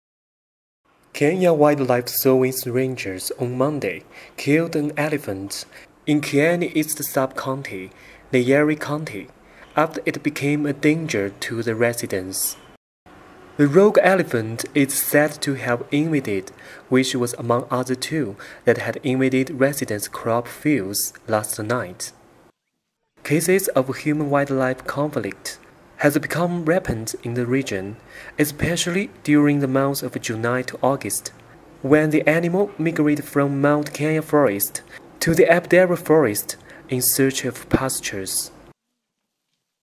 Stray elephants wreck havoc in an African village
Kenya wildlife service rangers on Monday  killed an elephant in Kieni East Sub-county, Nyeri County, Kenya after it became a danger to the residents.